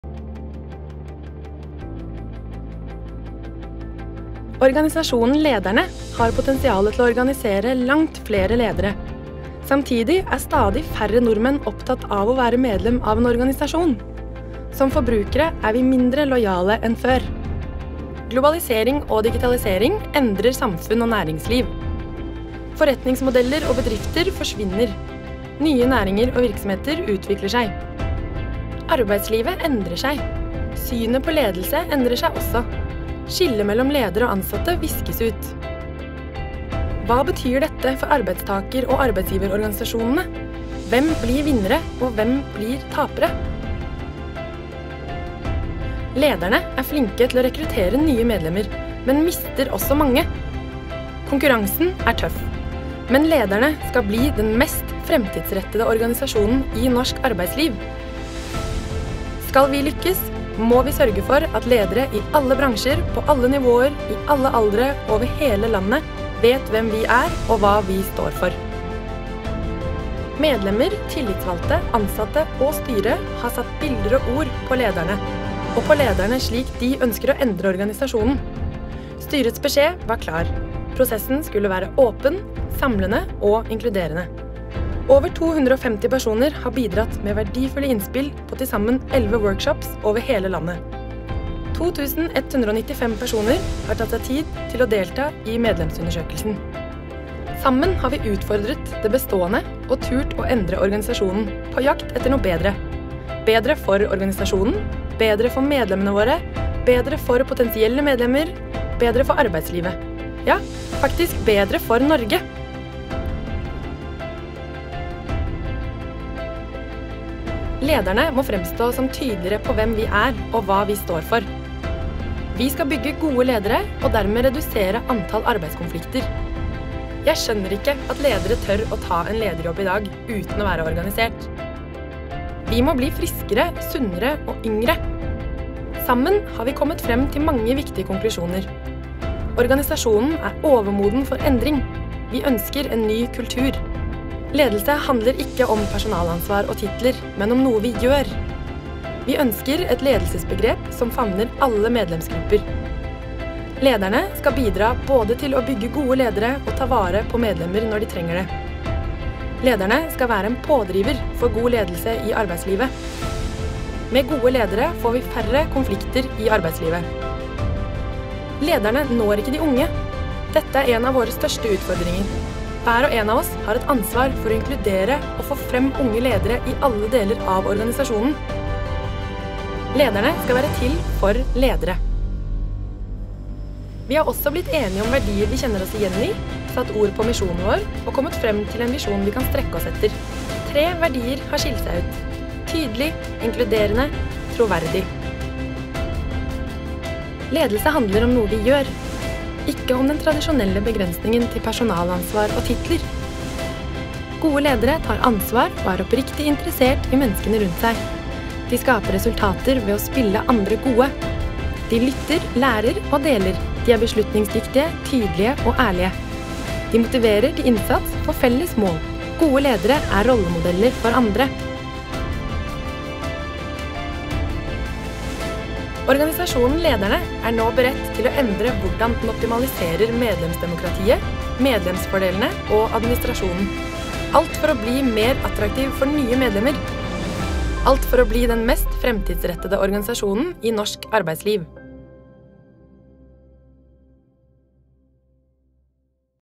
Ledernes strategiprosess - Vi bygger gode ledere
Hvordan endrer og moderniserer man en over 100 år gammel arbeidtakerorganisasjon? Bli med på reisen og se resultatet av strategiprosessen som arbeidstakerorganisasjonen Lederne gjennomfører i 2017, 2018 og 2019.